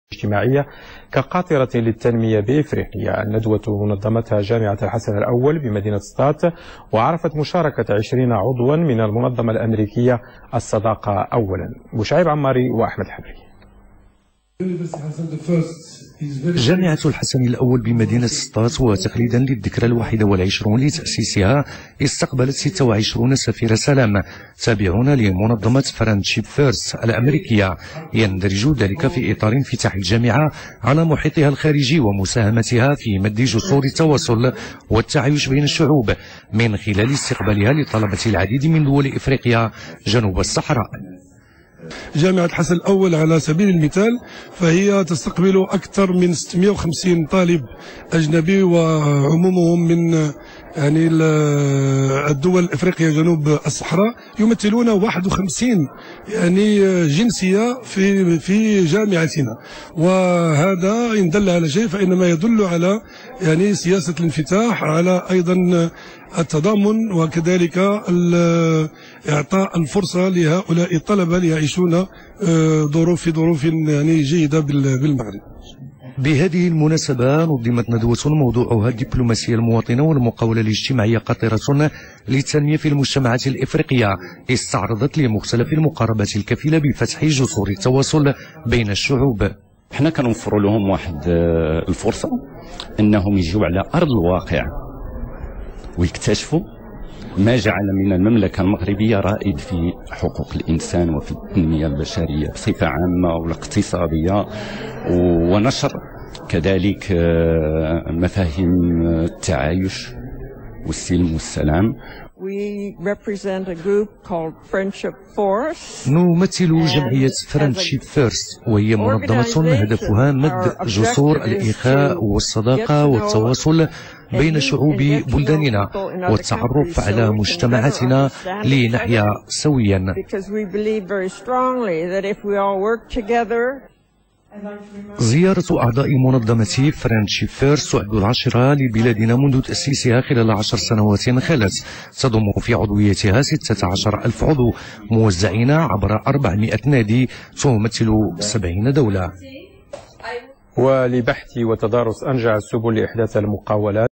aloula tv
April 2017.
International Peace Ambassadors visiting Morocco.
Invitation and program supervised by Lotfi Lamrani et Karim El Khyatti.